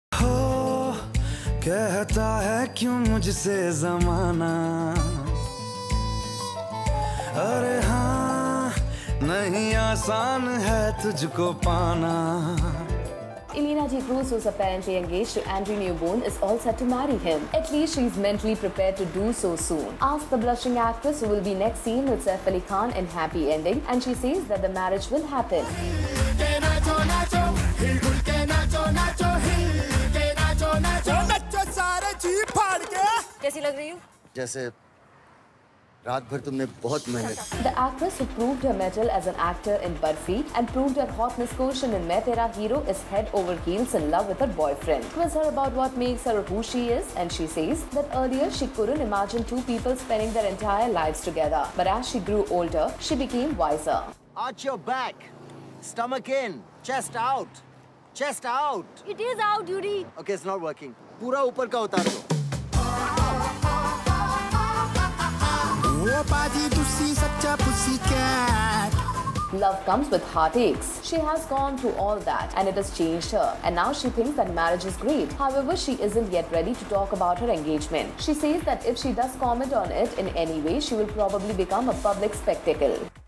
Is Ileana ready to exchange vows | Bollywood Masala | Latest Bollywood News
Ileana Dcruz who is apparently engaged to Andrew Kneebone is all set to marry him –at least she is mentally prepared to do so soon. Ask the blushing actress, who will next be seen with Saif Ali Khan in Happy Ending and she says, Marriage will happen.